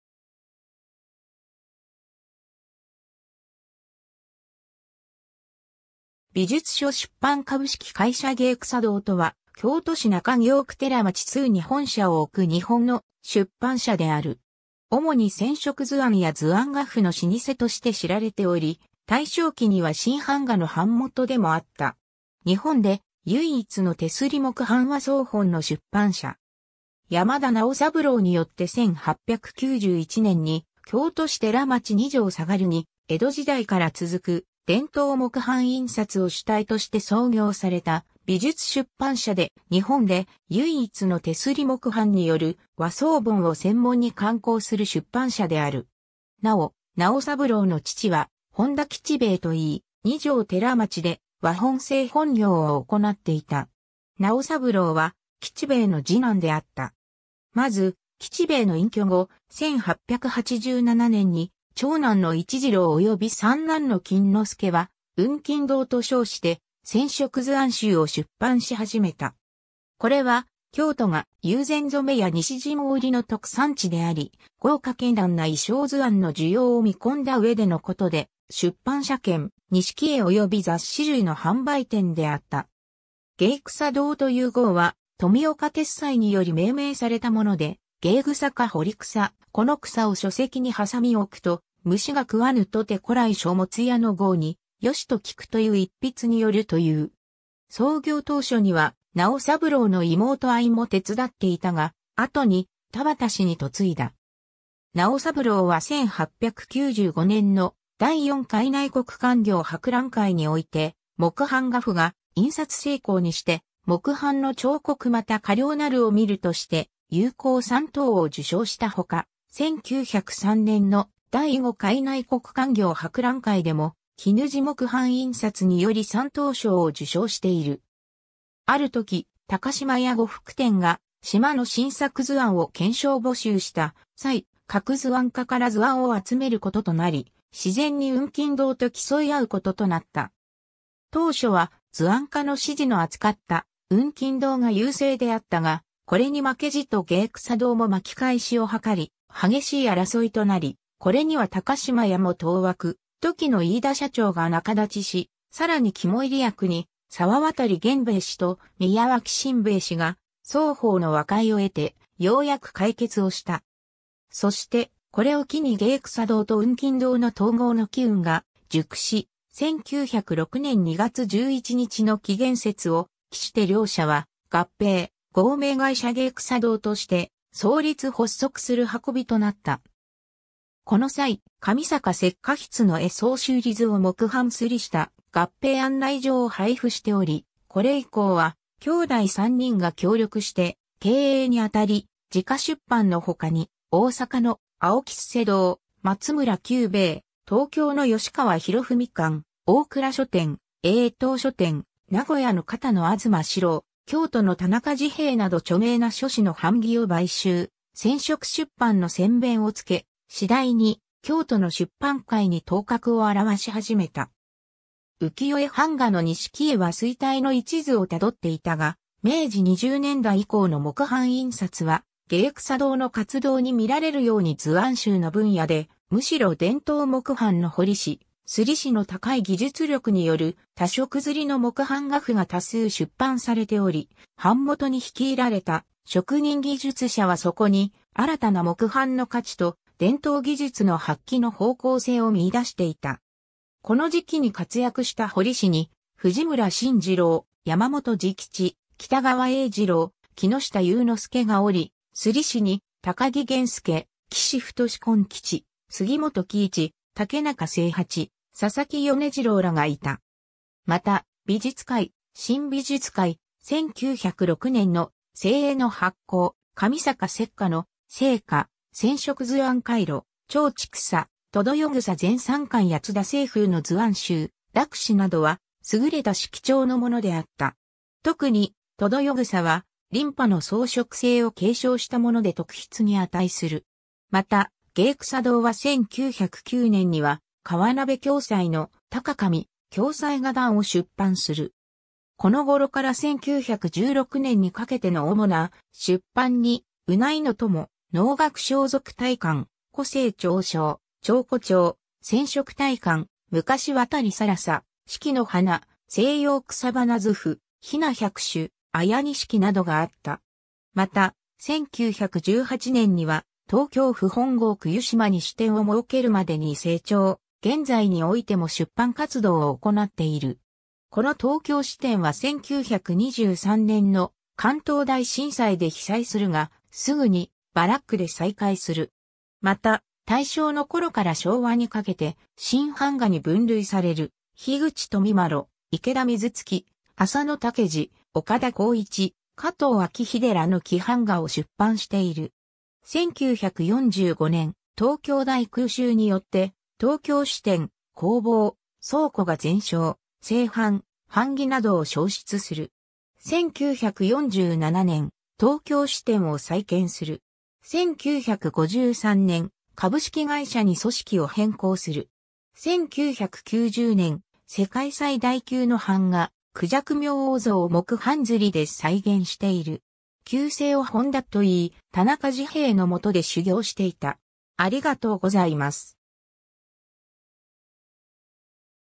芸艸堂
美術書出版株式会社芸艸堂（びじゅつしょしゅっぱんうんそうどう）とは京都市中京区寺町通に本社を置く日本の出版社である。
主に染色図案や図案画譜の老舗として知られており、大正期には新版画の版元でもあった。
日本で唯一の手摺木版和装本の出版社。
 山田直三郎によって1891年（明治24年）に京都市寺町二条下ルに江戸時代から続く伝統木版印刷を主体として創業された美術出版社で、日本で唯一の手摺木版による和装本を専門に刊行する出版社である。
なお、直三郎の父は本田吉兵衛といい、二条寺町で和本製本業を行っていた。
直三郎は吉兵衛の次男であった。
まず、吉兵衛の隠居後、1887年（明治20年）に長男の市次郎及び三男の金之助は雲錦堂と称して染色図案集を出版し始めた。
これは京都が友禅染や西陣織の特産地であり豪華絢爛な意匠図案の需要をみこんだ上でのことで出版社兼錦絵及び雑誌類の販売店であった。
 芸艸堂という号は富岡鉄斎により命名されたもので、「芸艸かほりくさ此艸を書籍にはさみおくと虫が喰わぬとて古来書物屋の号によしと聞く」と云う一筆によるという。
創業当初には直三郎の妹あいも手伝っていたが、後に田畑氏に嫁いだ。
直三郎は1895年（明治28年）の第4回内国勧業博覧会において、木版画譜が「印刷精巧にして木版の彫刻亦佳良なるを見る」として有功三等を受賞した他、1903年（明治36年）の第5回内国勧業博覧会でも「絹地木版印刷」により三等賞を受賞している。
ある時、高島屋呉服店が「縞」の新作図案を懸賞募集した際、各図案家から図案を集めることとなり、自然に雲錦堂と競い合うこととなった。
当初は図案家の支持の篤かった雲錦堂が優勢であったが、これに負けじと芸艸堂も巻返しを図り、激しい争いとなり、これには高島屋も当惑、時の飯田社長が仲立ちし、さらに肝煎役に沢渡源兵衛氏と宮脇新兵衛氏が双方の和解を得て漸く解決をした。
そして、これを機に芸艸堂と雲錦堂の統合の機運が熟し、1906年（明治39年）2月11日の紀元節を期して両社は合併、合名会社芸艸堂として創立発足する運びとなった。
この際、神坂雪佳筆の絵草紙売り図を木版摺りした合併案内状を配布しており、これ以降は兄弟3人が協力して経営に当たり、自家出版の他に大阪の青木嵩山堂、松村九兵衛、東京の吉川弘文館、大倉書店、永東書店、名古屋の片野東四郎（永楽堂）、京都の田中治兵衛（文求堂）など著名な書肆の版木を買収、染色出版の先鞭をつけ、次第に京都の出版界に頭角を現し始めた。
 浮世絵版画の錦絵は衰退の一途をたどっていたが、明治20年代以降の木版印刷は芸艸堂の活動にみられるように図案集の分野でむしろ伝統木版の彫師、摺師の高い技術力による多色摺の木版画譜が多数出版されており、版元に率いられた職人技術者はそこに新たな木版の価値と伝統技術の発揮の方向性を見出していた。
この時期に活躍した彫師に藤村新治郎、山本次吉、喜多川英二郎、木下猶之助がおり、摺師に高木源助、岸太根吉、椙本喜一、竹中清八、佐々木米次郎らがいた。
また、『美術海』、『新美術海』、1906年の『精英』（古谷紅麟ら）の発行、神坂雪佳の『精華』（1894年から1895年）、『染色図案 海路』（1902年）、『蝶千種』（1904年）、『百々世草』全3巻（1909年から1910年）や津田青楓の図案集『落柿』（1906年）などは優れた色調のものであった。
特に『百々世草』は琳派の装飾性を継承したもので特筆に値する。
 また、芸艸堂は1909年（明治42年）には河鍋暁斎の『鷹かゞみ』、『暁斎画談』を出版する。
この頃から1916年（大正5年）にかけての主な出版に『うなゐの友』、『能楽装束大観』、『古制徴証』、『徴古帖』、『染色大鑑』、『昔渡更紗』、『四季の花』、『西洋草花図譜』、『雛百種』、『綾錦』などがあった。
また、1918年（大正7年）には東京府本郷区湯島に支店を設けるまでに成長、現在においても出版活動を行っている。
この東京支店は1923年（大正12年）の関東大震災で被災するが、すぐにバラックで再開する。
また、大正のころから昭和にかけて新版画に分類される樋口富麿、池田瑞月、浅野竹二、岡田行一、加藤晃秀らの木版画を出版している。
 1945年、東京大空襲によって東京支店、工房、倉庫が全焼、製版、版木などを焼失する。
1947年、東京支店を再建する。
1953年、株式会社に組織を変更する。
1990年...